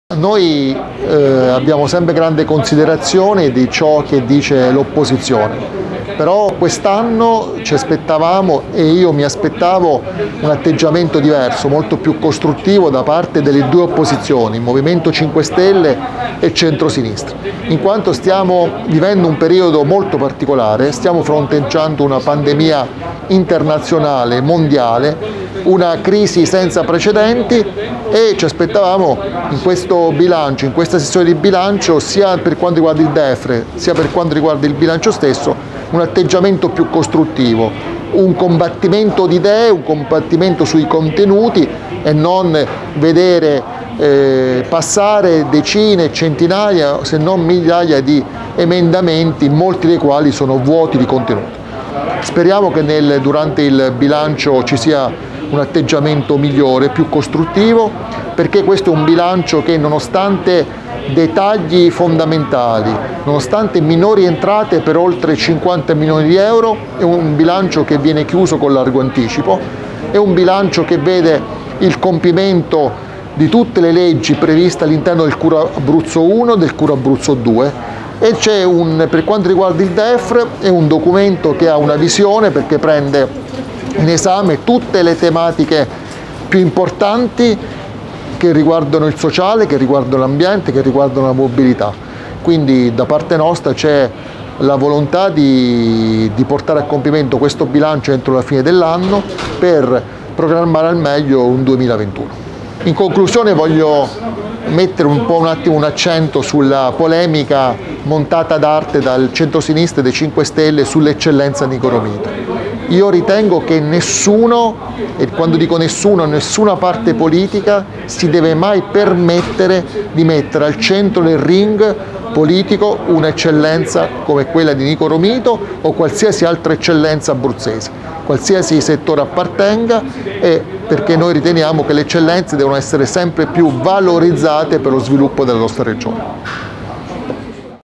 DEFR e Bilancio, il commento di Guerino Testa
Le considerazioni del Capogruppo di FdI Guerino Testa